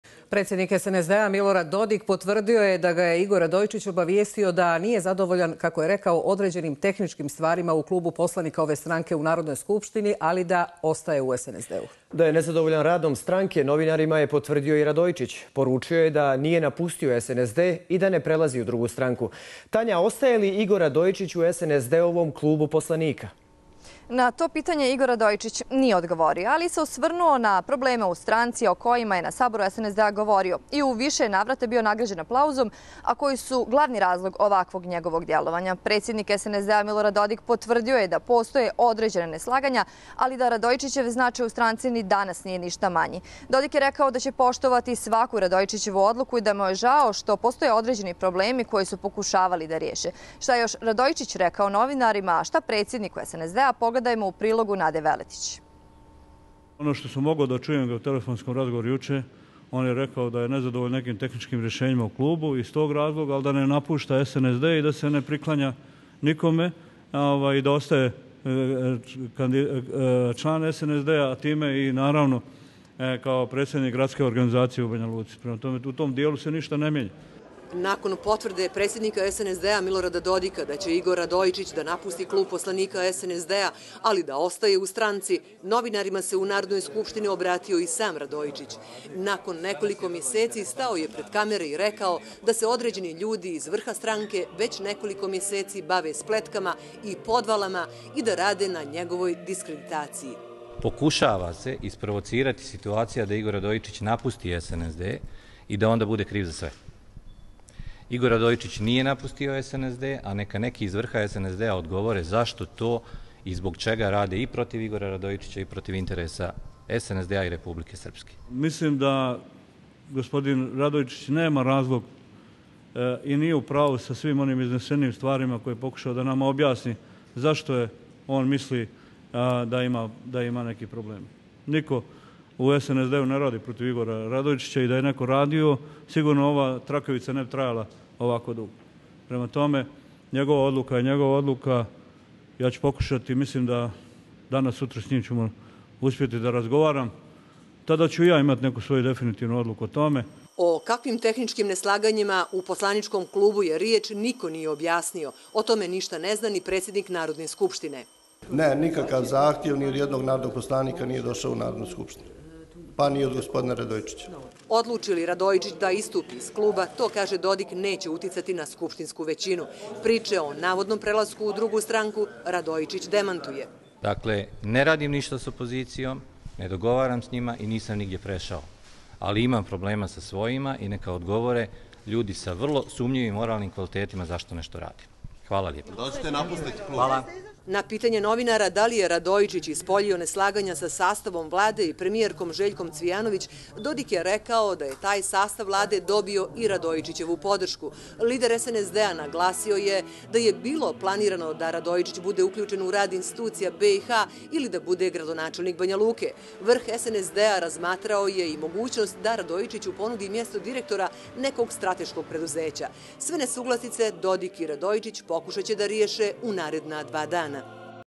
Radojičić: Nisam napustio SNSD
Poslanik SNSD-a u Narodnoj skupštini Republike Srpske Igor Radojičić izjavio je da nije napustio SNSD i da nigdje nije prešao.